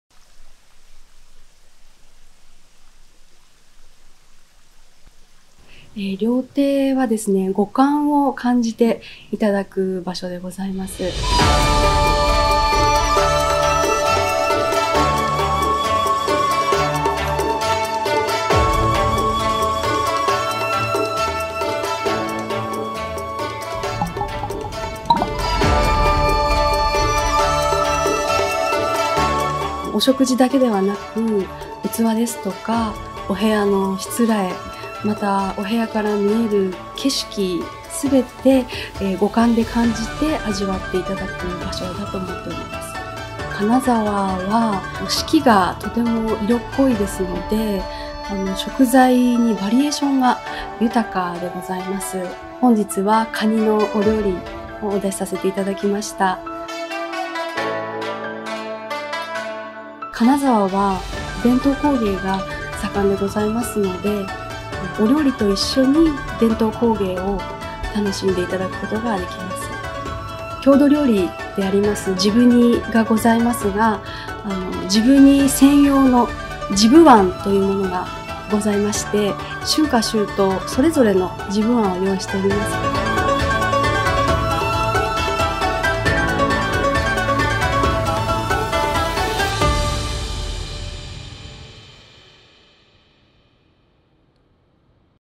RYOTEI -Peak Hospitality-
A culmination of Japanese essential culture. Step into a space of pure Japanese cuisine and hospitality in the peak of full bloom during one of Kanazawa’s rich 4 seasons.

Click to see more authentic Japanese artistry still alive in Kanazawa via this 10-part, short film collection

日本文化の集大成である料亭。四季が色濃い金沢で花開いた最高峰のおもてなし空間がここにあります。

金沢に残る本物の技を特集した10本の動画はこちらからご覧いただけます